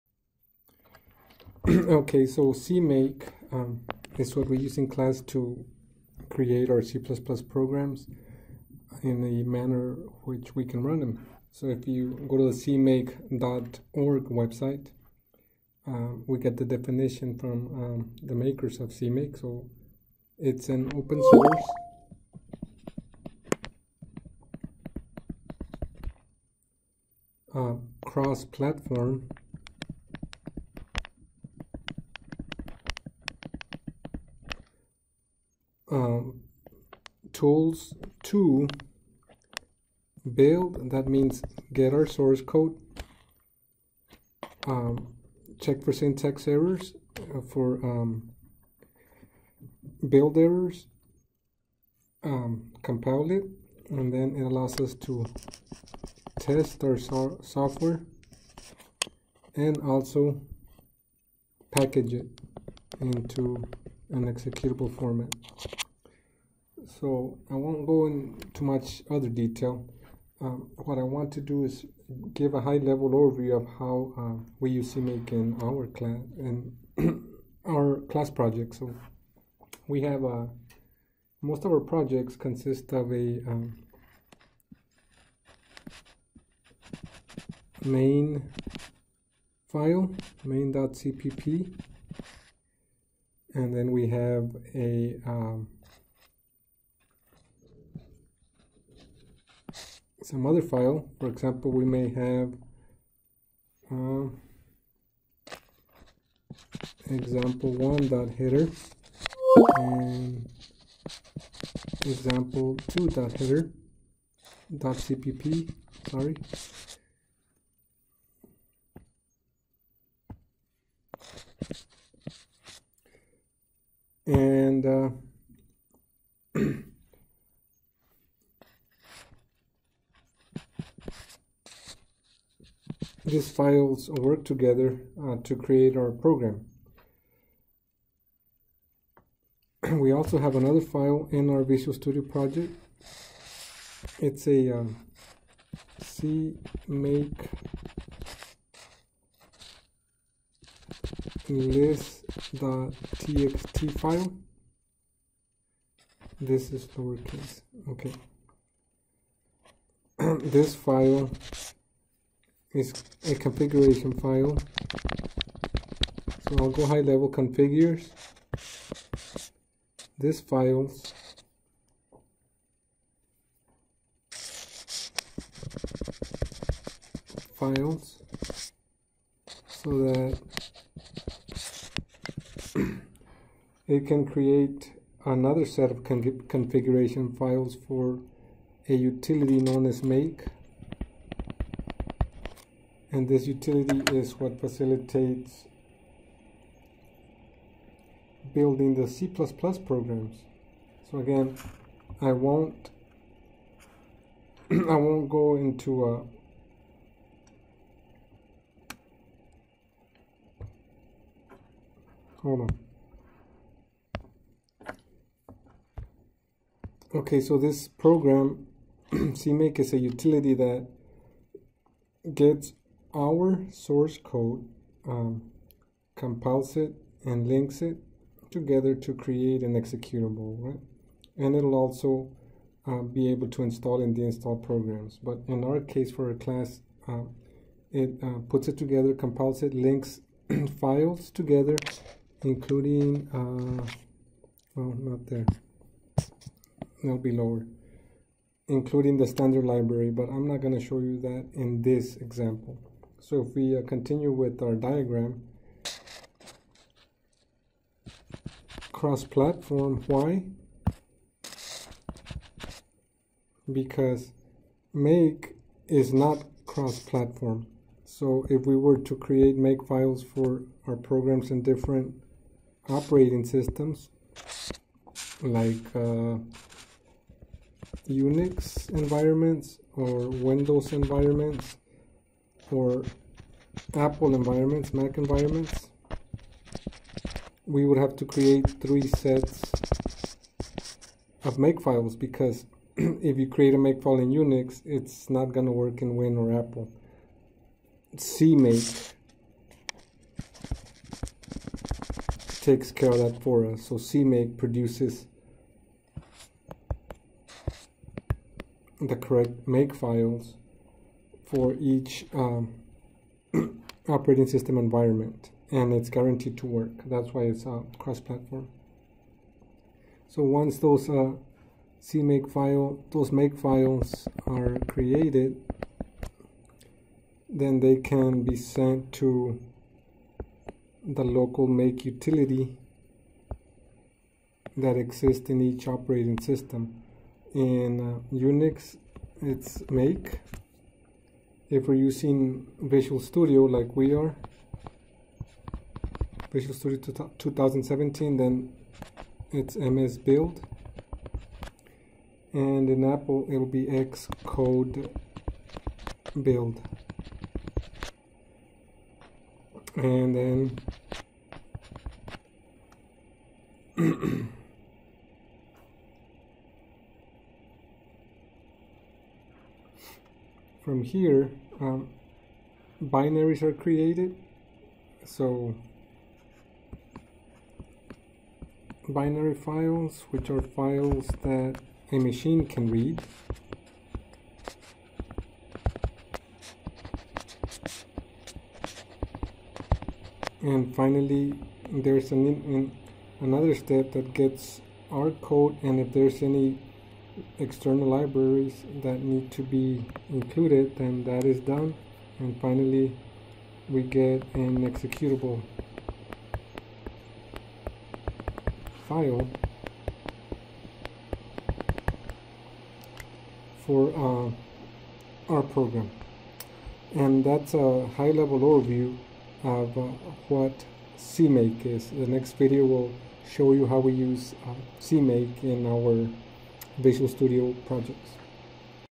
c++ CMake
High level overview of the CMake build utility